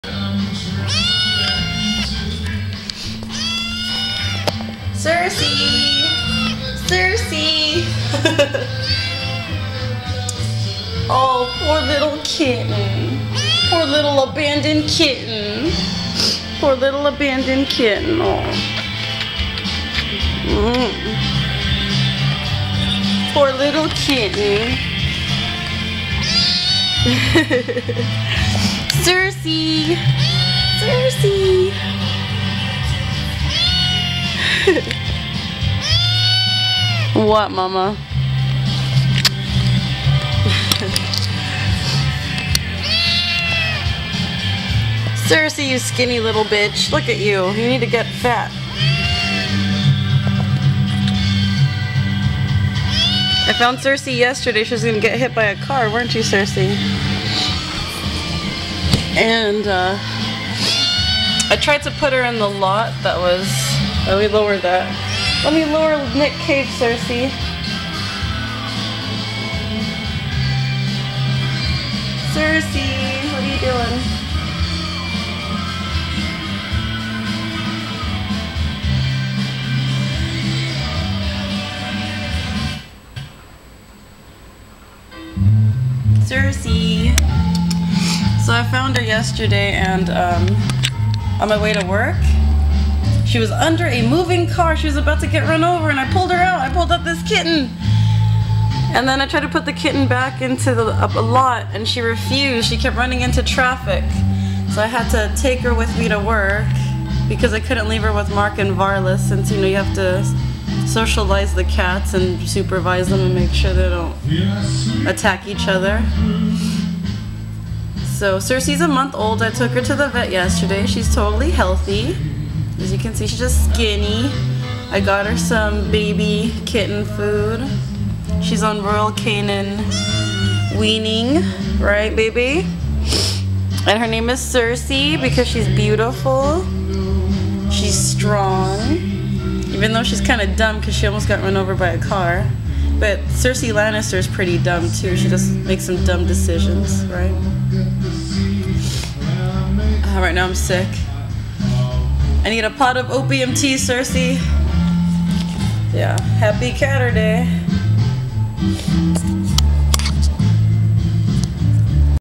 Caturday: Finding One Month Old Cersei
Found a kitten under a moving car on my way to work on Friday.

Typically, you should leave any healthy kittens in a safe location where you found them so their mother can take care of them. But since Cersei kept running into traffic and she was cute, I decided to take her in. So I have a 3rd cat but that's it. No more cats!